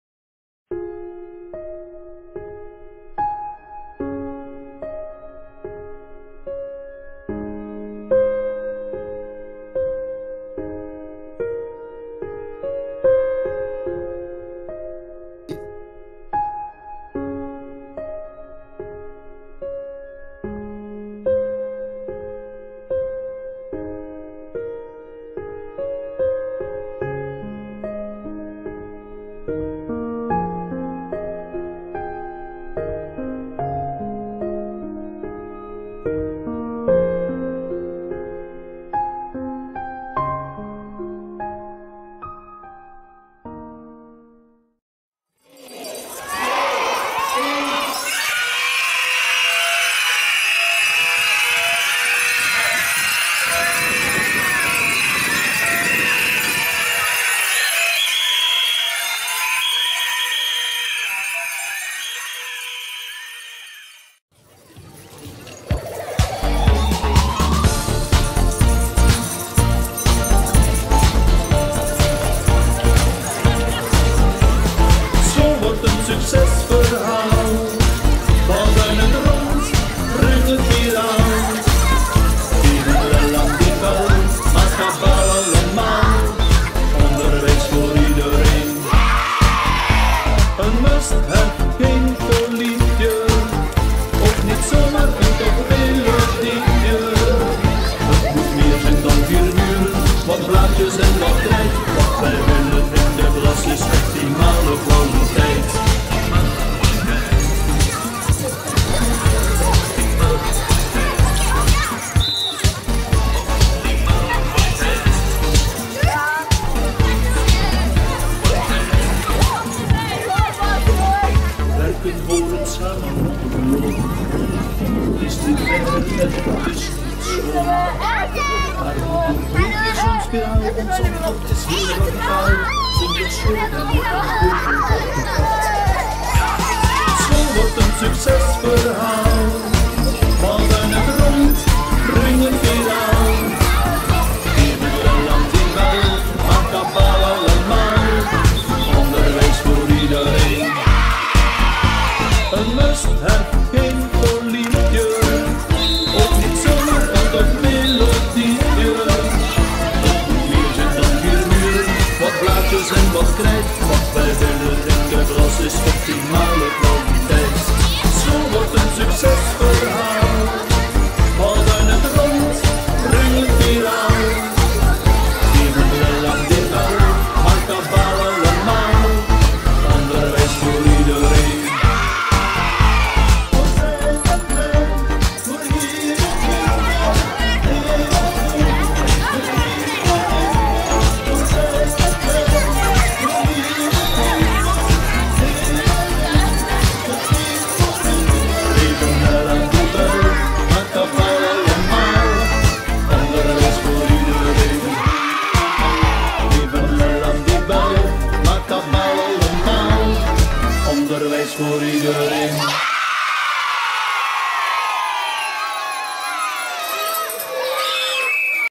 SAB Nieuws - Saved By The Bell '19
Op zaterdag 5 oktober is het "Dag van de leerkracht". Op die dag is er ook elk jaar de "Saved By The Bell" actie, een oproep om lawaai te maken omdat iedereen wakker moet worden: wereldwijd heeft nog niet iedereen het geluk om de schoolbel te horen rinkelen. Meer dan 262 miljoen kinderen en jongeren genieten geen onderwijs. Daar willen wij verandering in brengen. Omdat 5 oktober in het weekend valt, maakten wij lawaai en dansten we op vrijdag. Het SAB Nieuw laat je meegenieten.